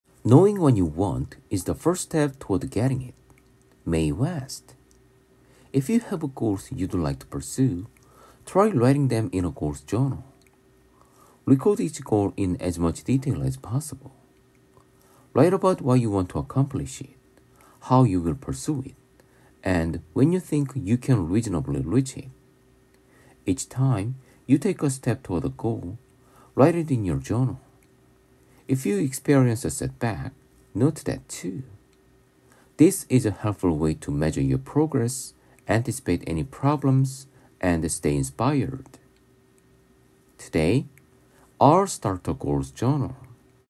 20200301
“Knowing what you want is the first step toward getting it.”
Mae West

If you have goals you’d like to pursue, try writing them in a Goals Journal.
Record each goal in as much detail as possible
Write about why you want to accomplish it, how you’ll pursue it, and when you think you can reasonably reach it.
Each time you take a step toward a goal, write it in your journal.
If you experience a setback, note that, too.
This is a helpful way to measure your progress, anticipate any problems, and stay inspired.

Today
I’ll start a Goals Journal.